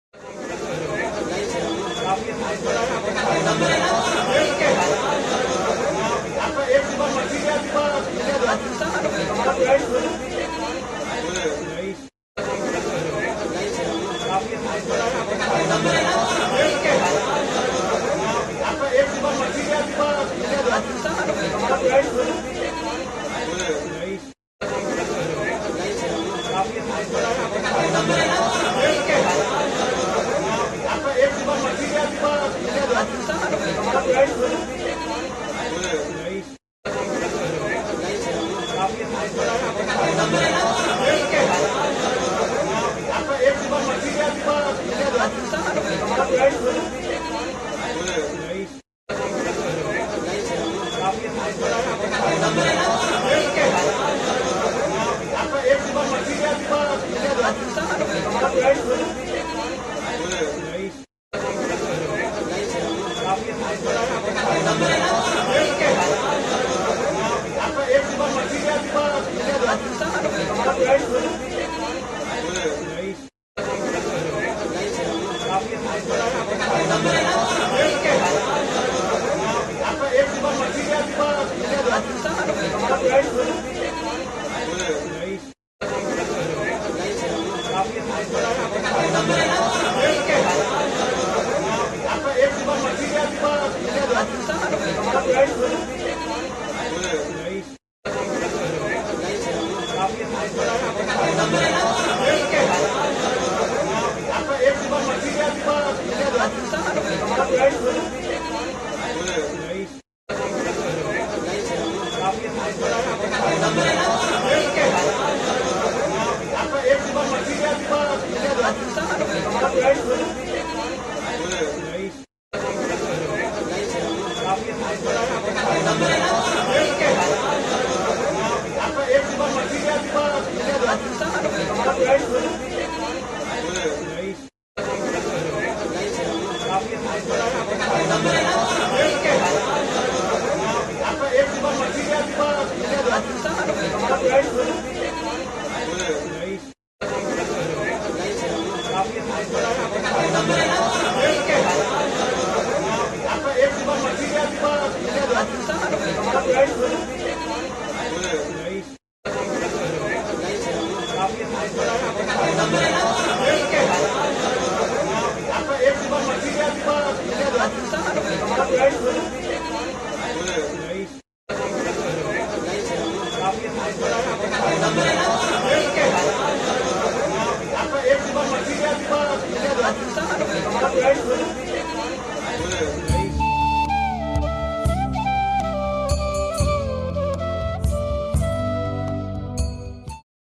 কোনে কিমান খাব পাৰে।
হোলীৰ প্ৰাৰ্কক্ষণত লক্ষ্ণৌত 'বাহুবলী গুজিয়া' খোৱা প্ৰতিযোগিতা।